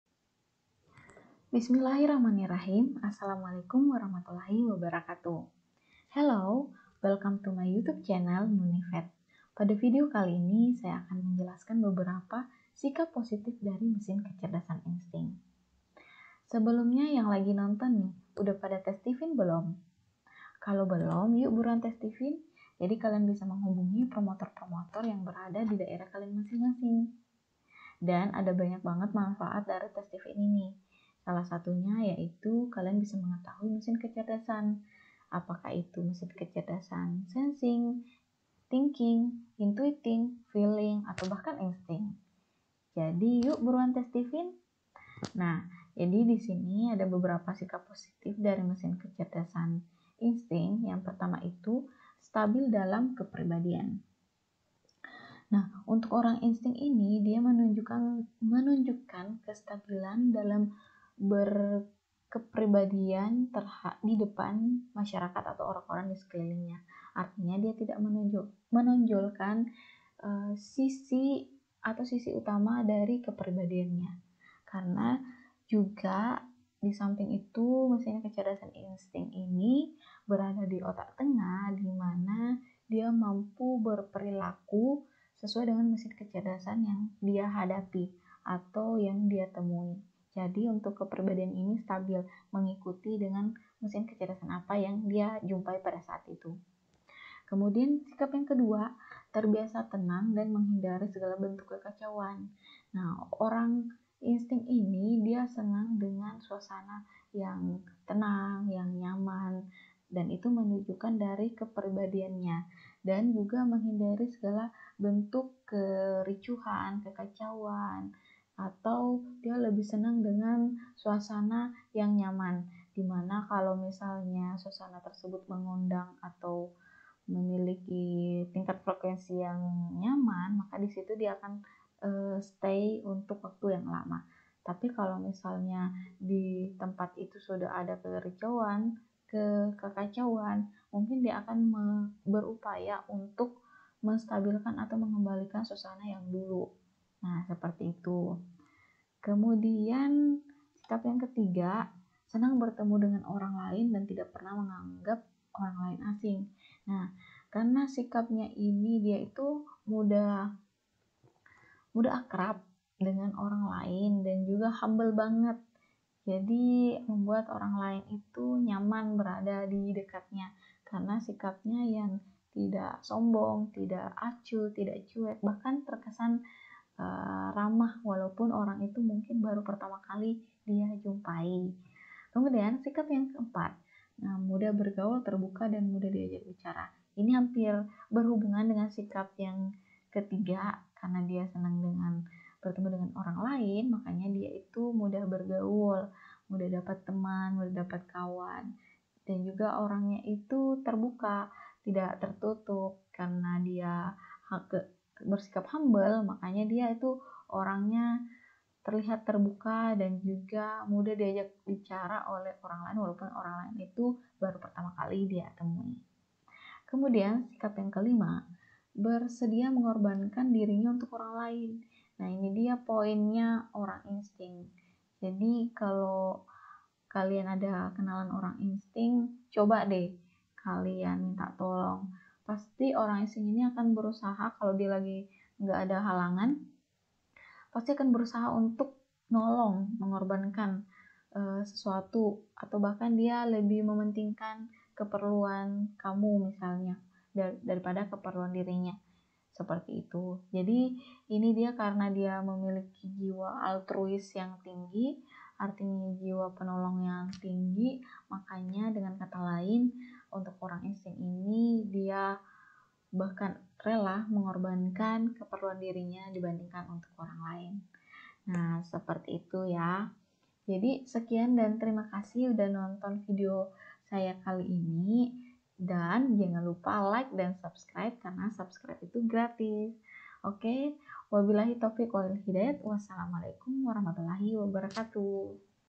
Sikap Positif MK. Insting - Tes STIFIn
STIFIn adalah suatu konsep ilmu yang memetakan manusia berdasarkan system operasi otaknya. Dari berbagai teori yang ada, STIFIn membaginya menjadi lima bagian. Yang pertama sensing, thinking, intuiting, feeling, dan insting. Dalam konsep STIFIn kelima hal itu tergolong mesin kecerdasan.

Bisa hubungi saya di kontak : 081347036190
Untuk daerah yang bisa dijangkau : Bulukumba​​​​​​, Bone​​​​​​, Takalar​